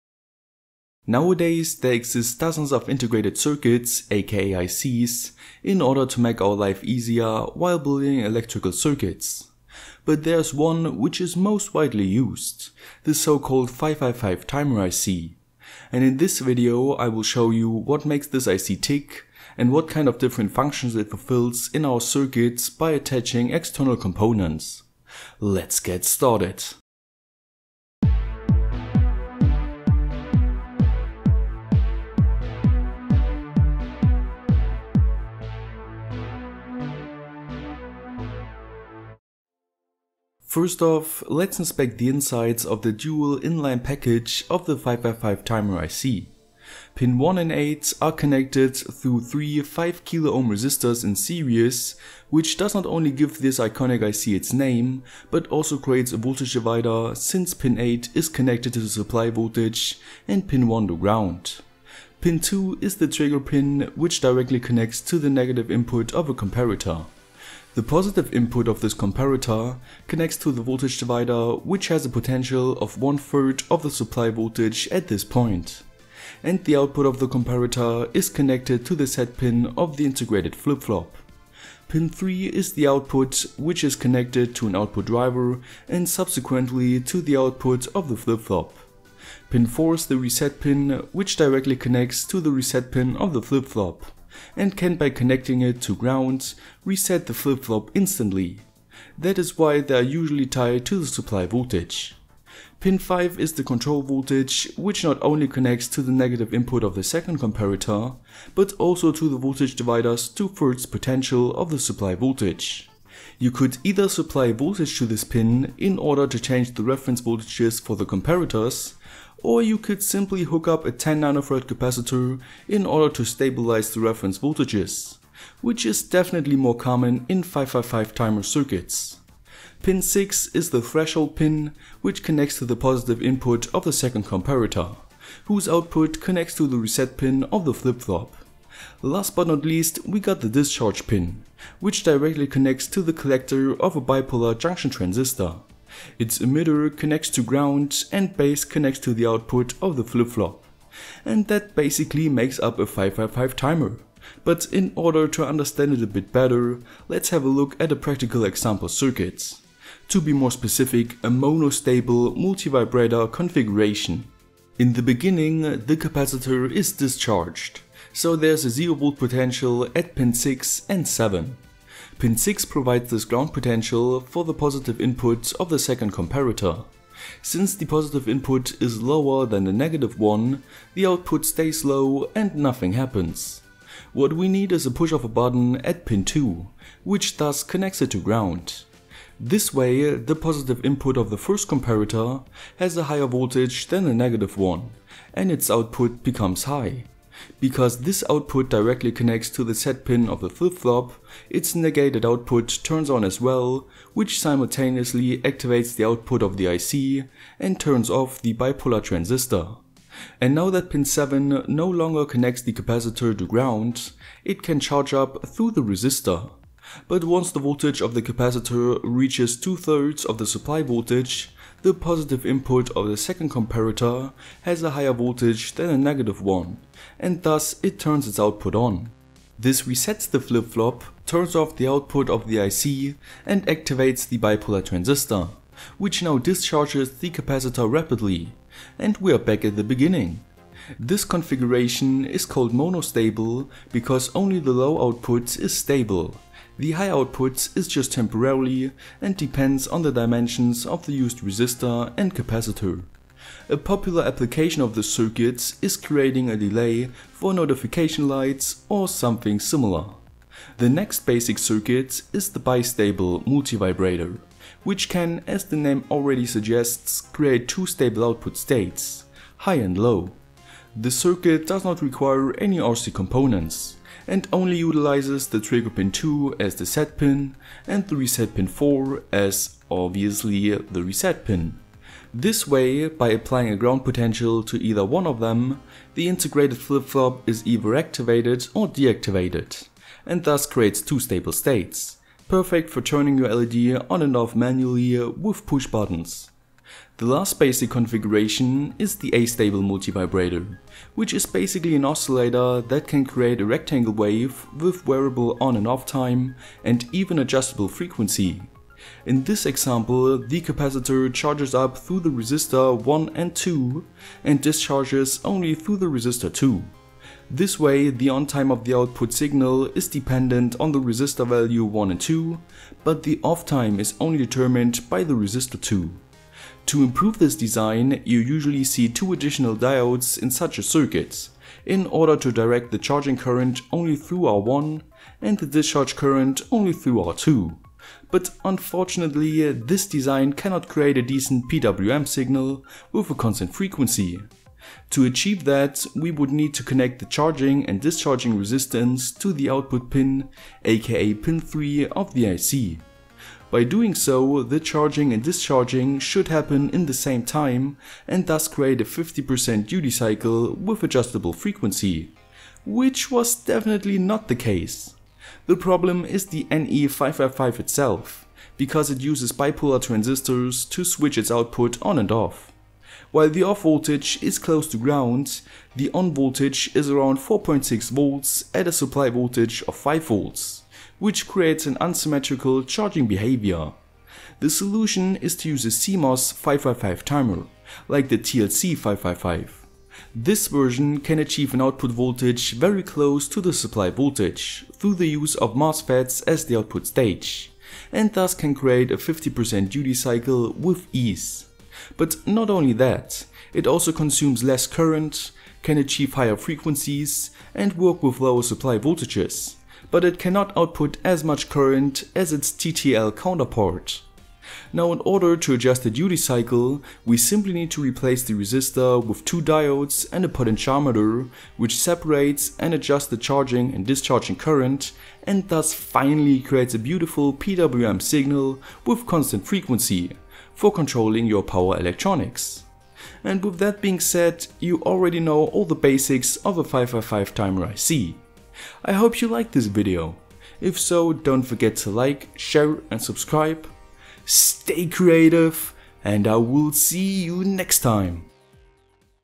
Electronic Basics #26: 555 Timer IC
NE555 (TTL)
TLC555 (CMOS)
NE555 (TTL)
TLC555 (CMOS)
Amazon.de:
NE555 (TTL)
TLC555 (CMOS)

In this episode of Electronic Basics I will show you what make the popular 555 Timer IC tick and how we can use it in our circuits as a monostable, bistable and astable multivibrator in order to create delays or even PWM Signals.